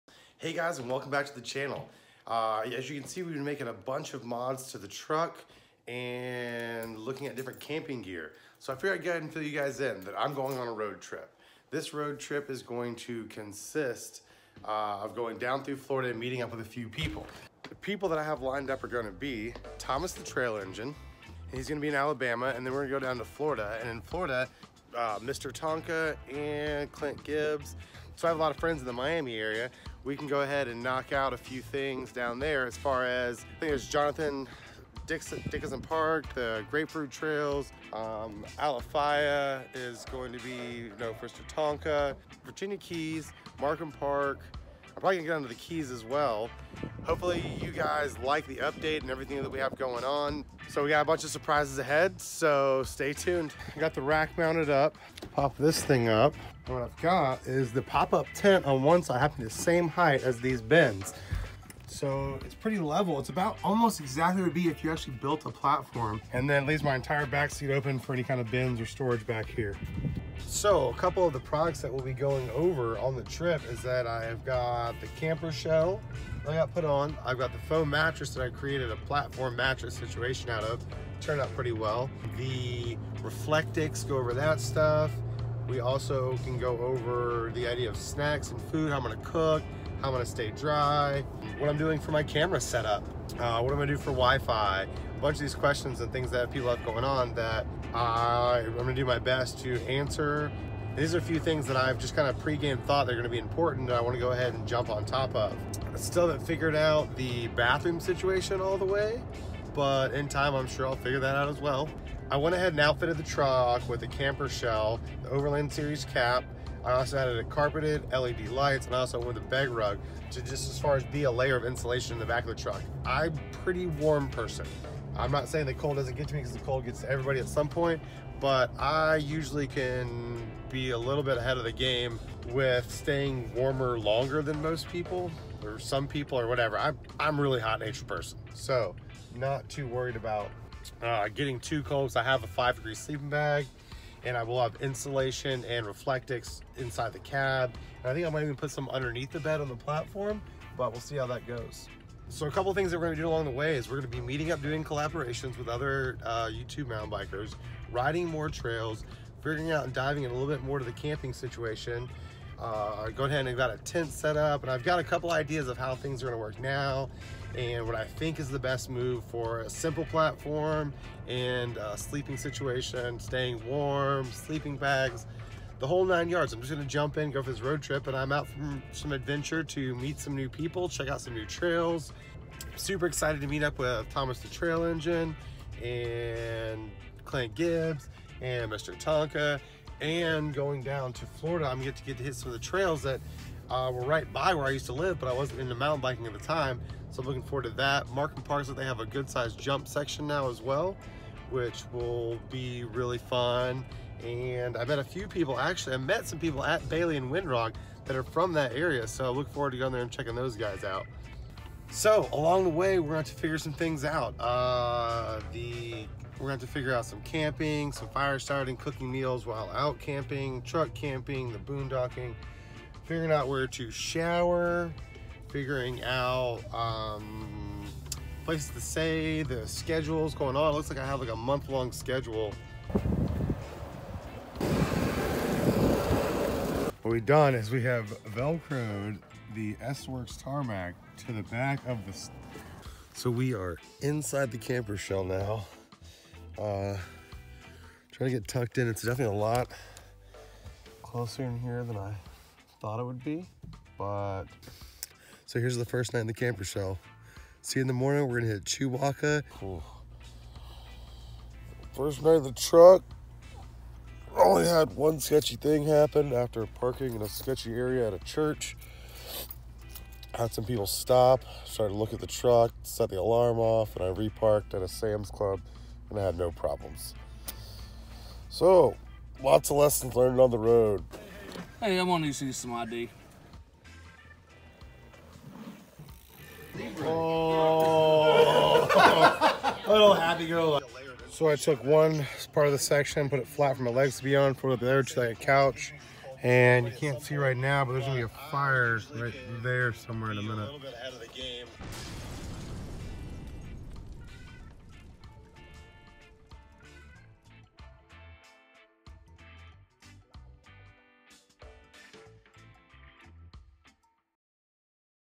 Truck Camping Trip
Welcome back to the channel. We are going on a road trip. We are meeting up with Mr.Tonka, Clint Gibbs and Thomas the Trail engine.

We will be taking the Tacoma on the road explaining the Overland series truck cap, the bike rack, camping, cooking, off roading, Sleeping bag, foam mattress and who on.

If you want to help get me out to the west cost or far north
 here is the way

And if you want to keep up day to day stuff check out IG I'm every where